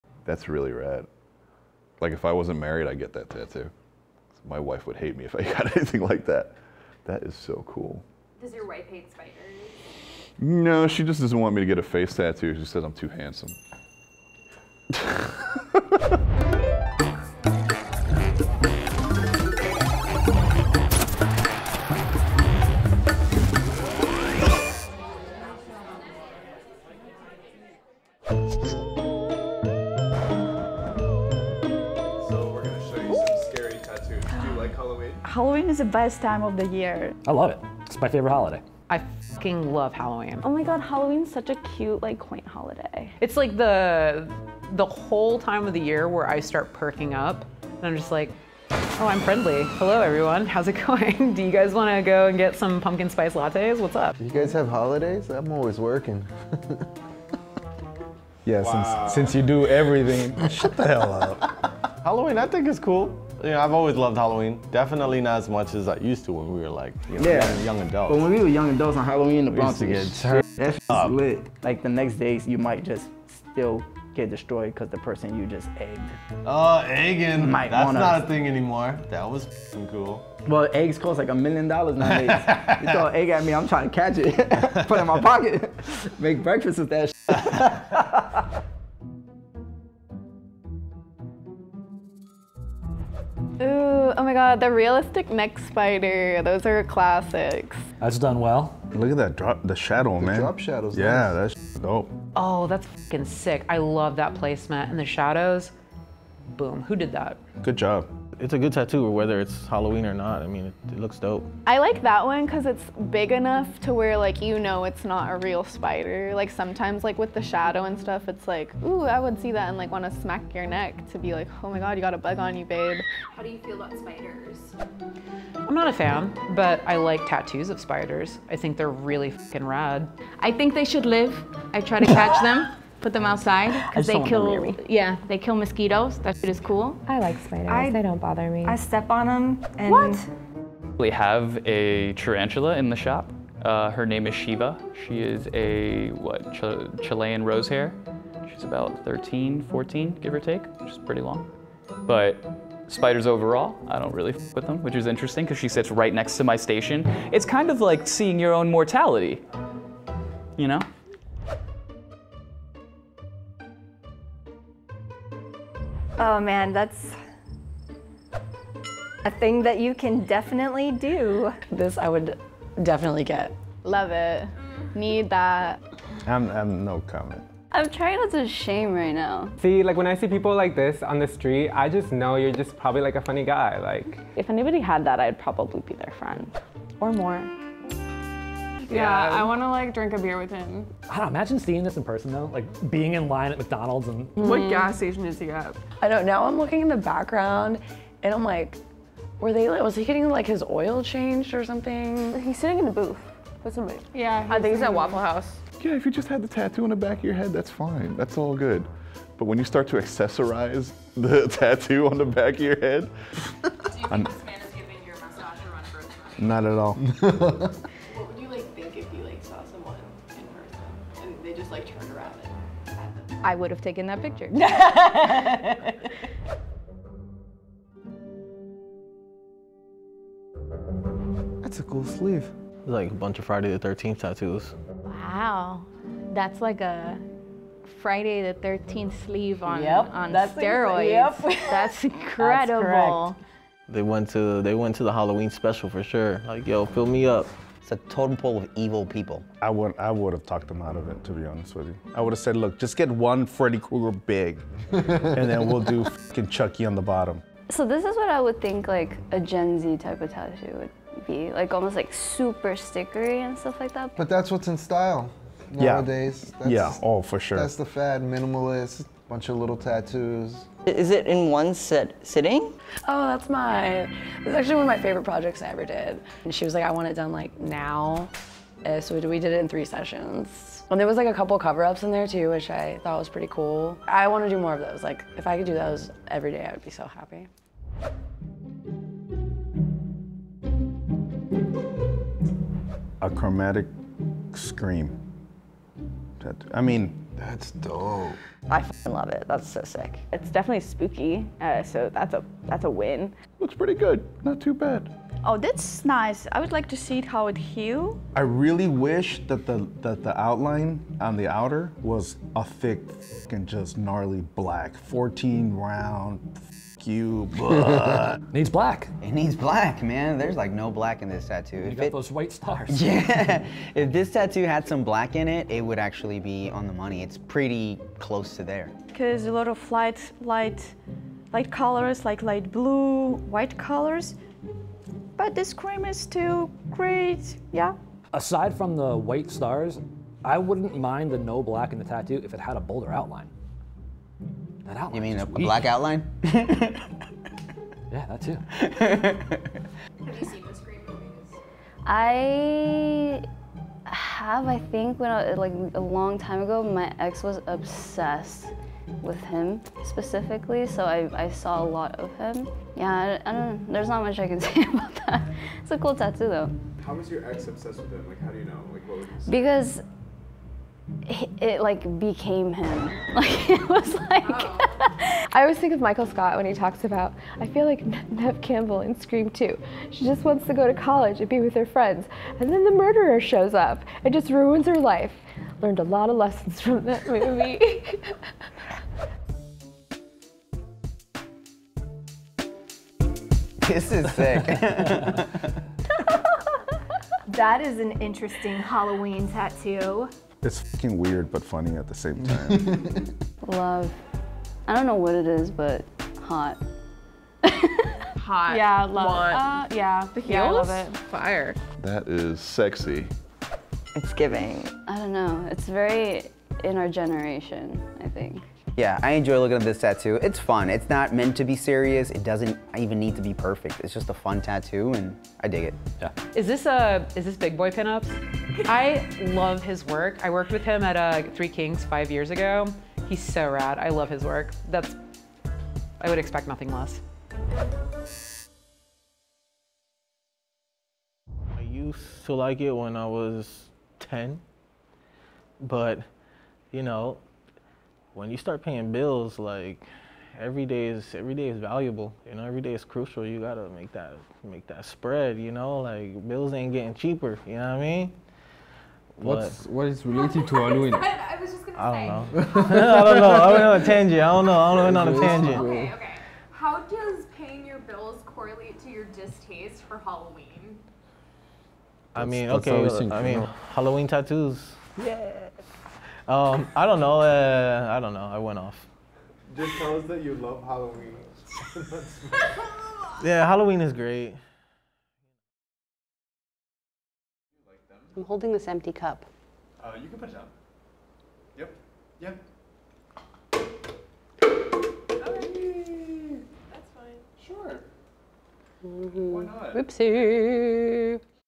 'That's A Friday The Thirteenth Sleeve On Steroids' | Halloween Tattoos | Tattoo Artists React
Happy Halloween! It's our favorite time of the year, so we couldn't help but show our panel of tattoo artists some of the spookiest tattoos out there - from an oddly attractive Michael Myers to chrome Ghostface, we have it all. Watch if you dare.

Directed & Edited by Sammy Martel
Producers — Sami Hajar, Sammy Martel
Associate Producers — Julia Cancilla, Mina Eren Ozgu